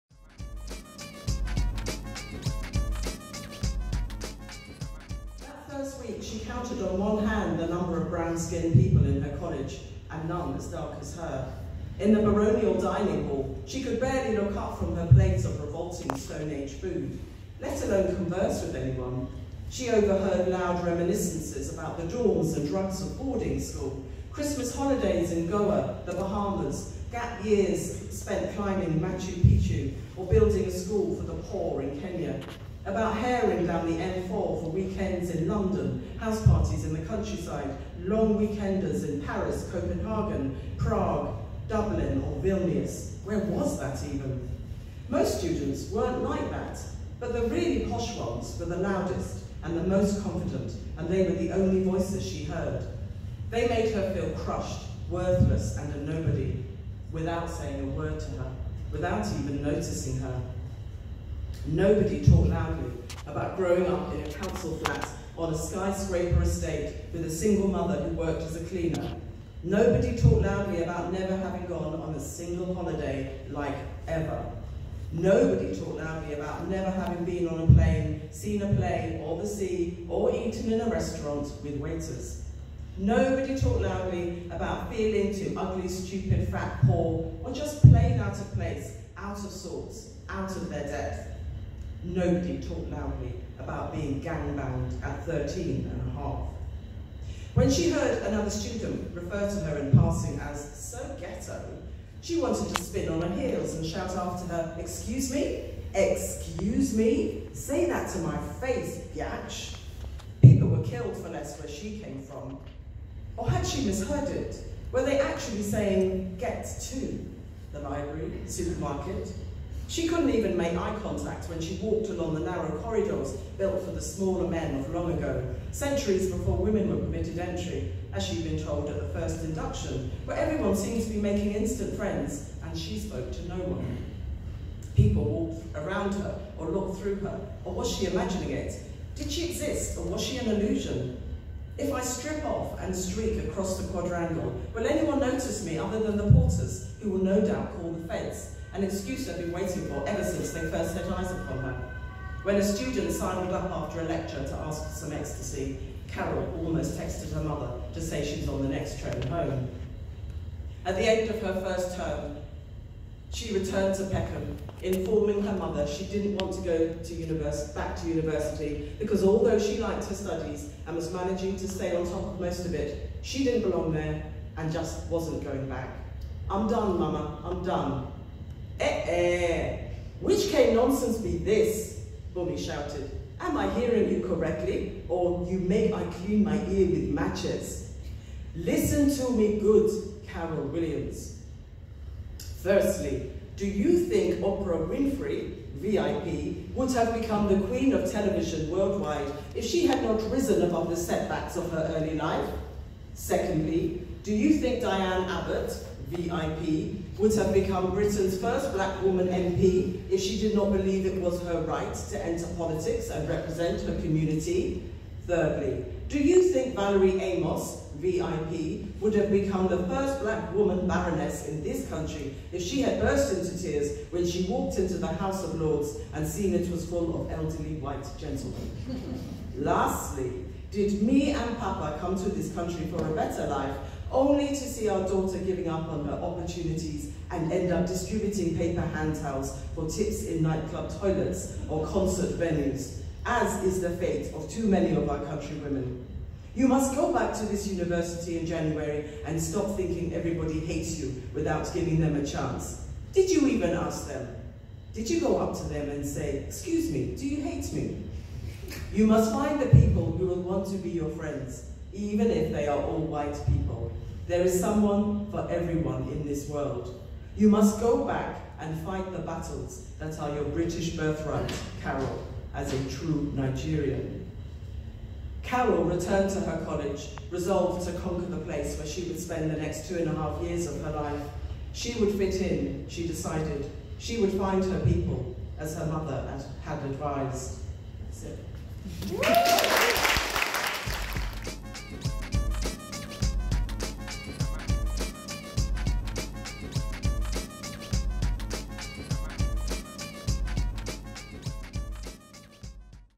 Bernadine Evaristo reads from "Girl, Woman, Other". Afropean @ The Holland Festival
2019 Booker prize nominee Bernardine Evaristo reads from her latest novel at "The Welcome Table" hosted by Afropean at Holland Festival 2019